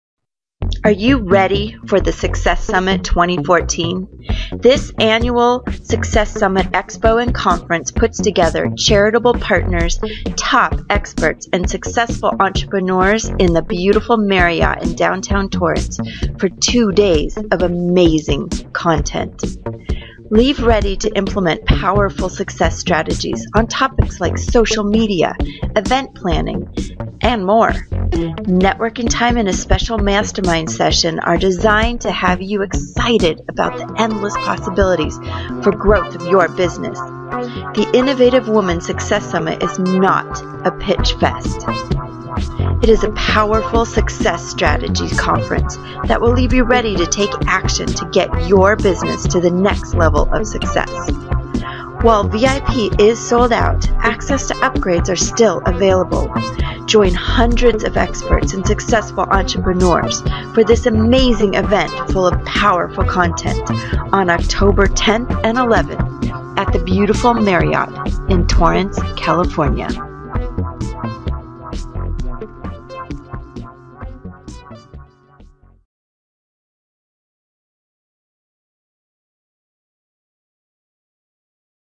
Success Summit Expo and Conference 2014 Preview/Review Top Experts- Successful Entrepreneurs
Are you ready for the success summit 2014?
This annual success summit expo and conference puts together charitable partners, top experts and successful entrepreneurs in the beautiful Marriott for 2 days of amazing content.
Leave ready to implement powerful success strategies on topics like social media, event planning, and more. Networking time and a special mastermind session are designed to have you excited about the endless possibilities for growth of your business! The innovative woman success summit is not a pitch fest; it is a powerful success strategy conference that will leave you ready to take action to take your business to the next level of success. While VIP is sold out, access to upgrades are still available. Join hundreds of experts and successful entrepreneurs for this amazing event full of powerful content. October 10th and 11th, at the beautiful Marriott in Torrance , CA.